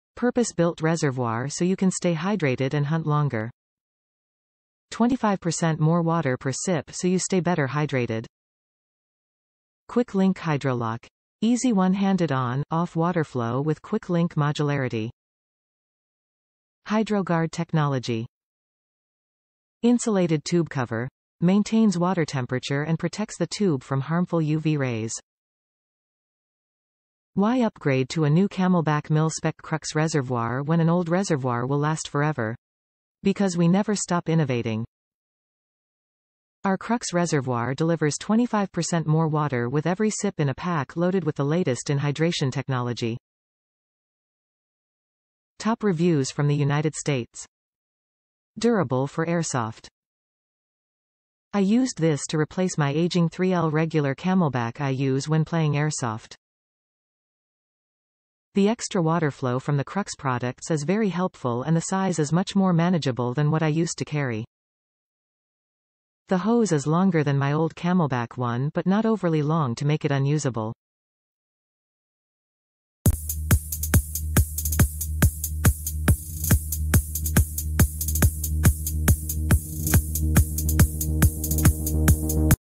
Camelbak - Mil-Spec Crux 50 oz/1.5L Accessory Reservoir (2053001015), Brown
Purpose-built reservoir so you can stay hydrated and hunt longer
25% more water per sip so you stay better hydrated
Quick Link Hydrolock: Easy one-handed on/off water flow with Quick Link modularity
Hydroguard Technology
Insulated Tube Cover: Maintains water temperature and protects the tube from harmful UV rays
Why upgrade to a new CamelBak Mil Spec Crux reservoir when an old reservoir will last forever? Because we never stop innovating. Our Crux reservoir delivers 25% more water with every sip in a pack loaded with the latest in hydration technology.

Durable for airsoft
I used this to replace my aging 3L regular camelbak I use when playing airsoft. The extra waterflow from the Crux products is very helpful and the size is much more manageable than what I used to carry. The hose is longer than my old camelbak one but not overly long to make it unusable